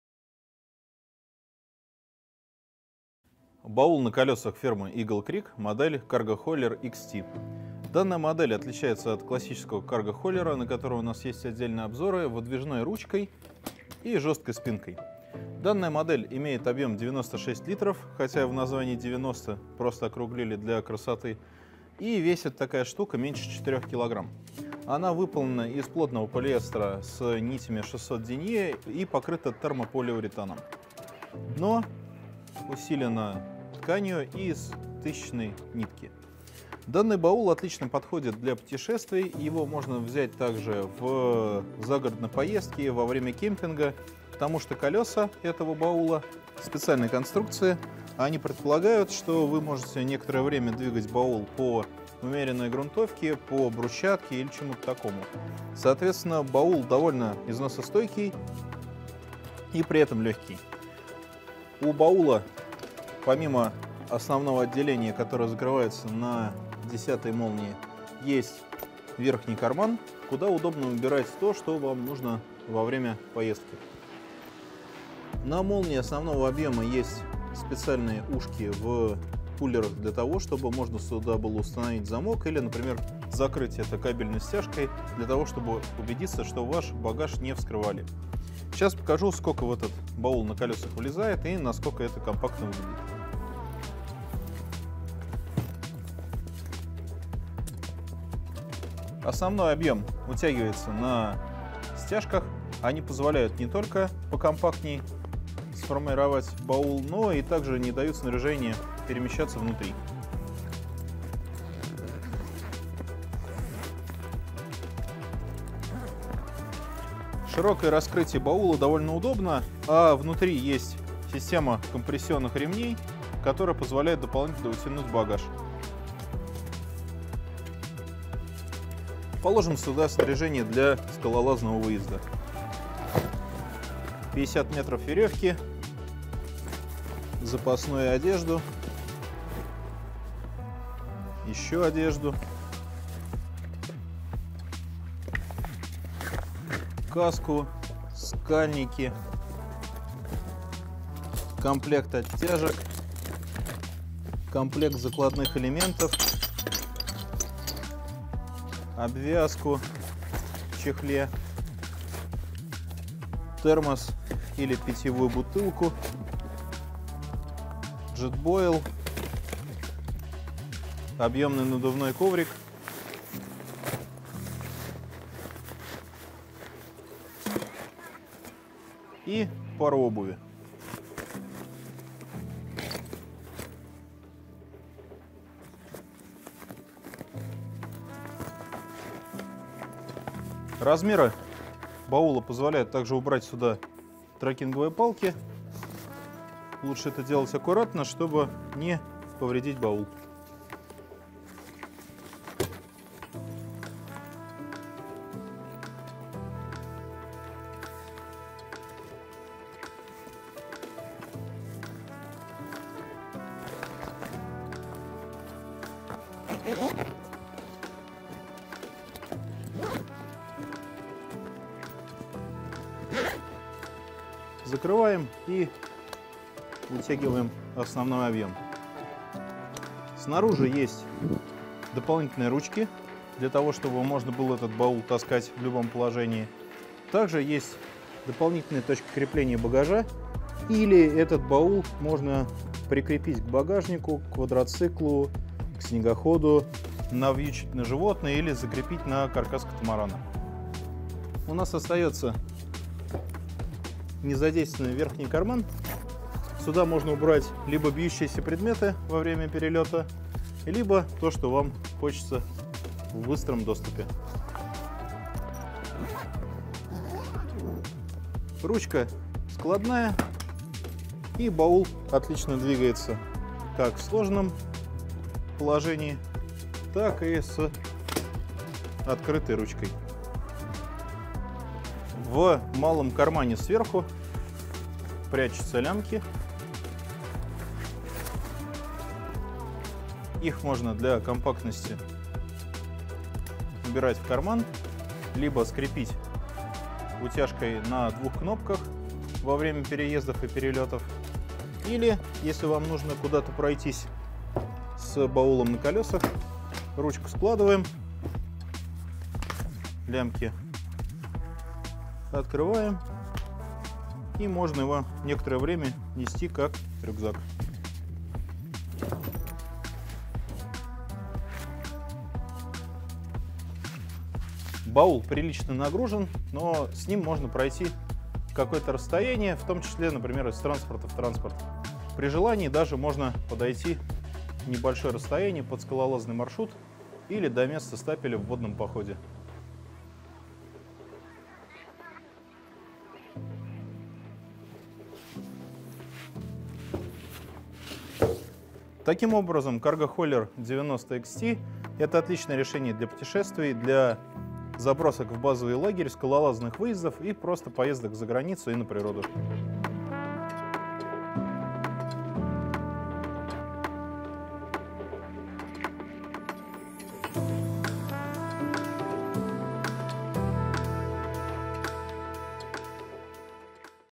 Баул Eagle Creek Cargo Hauler XT 90L. Обзор
Cargo Hauler Wheeled Duffel XT 90L – универсальный баул с большими колёсами, которые проедут по любой поверхности.
Баул оснащён лямками, поэтому вы сможете нести его как рюкзак, что значительно увеличит вашу скорость передвижения, а также вам не будут страшны многочисленные лестницы и бездорожье.
Cargo Hauler Wheeled Duffel XT предоставляет большой объём для хранения многочисленного снаряжения.
Прочный материал с водоотталкивающей пропиткой поможет вашей экипировке благополучно пережить все перемещения.

• водоотталкивающая ткань для максимальной защиты снаряжения;
• широкое U-образное раскрытие;
• прочные ручки для переноски;
• убирающиеся плечевые лямки;
• U-образное раскрытие обеспечивает лёгкий доступ к основному отделению;
• большие колёса добавляют ещё больше универсальности для лёгкого путешествия;
• телескопическая ручка;
• карман на молнии;
• усиленное дно защищает содержимое от ударов;
• прямоугольная форма отлично подходит для укладки с другим багажом;
• молнии, совместимые с кодовыми замками, защищены от попадания воды;
• молниями легко пользоваться в перчатках;
• светоотражающие пуллеры на бегунках молний;
• интегрируется с системой Pack-It компании Eagle Creek, чтобы вы могли оставаться организованными и максимально увеличить объём хранения в сумке.